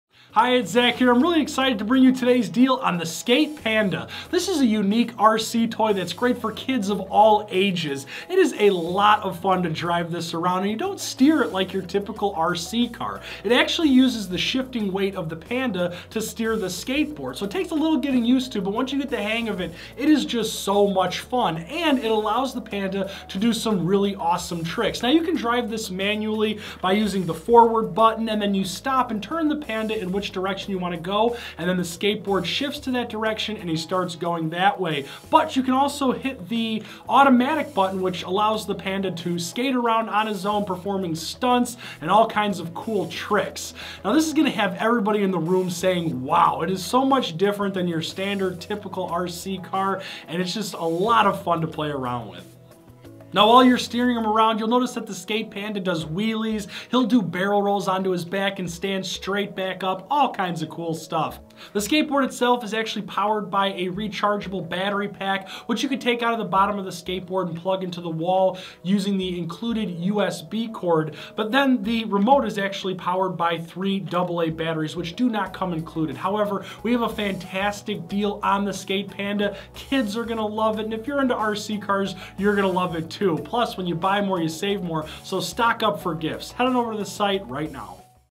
Remote Control Skate Panda
This action-packed Remote Control Skate Panda will WOW you and your kids with fascinating stunts, 360's, spins, wheelies, and more on the any floor or low profile carpet.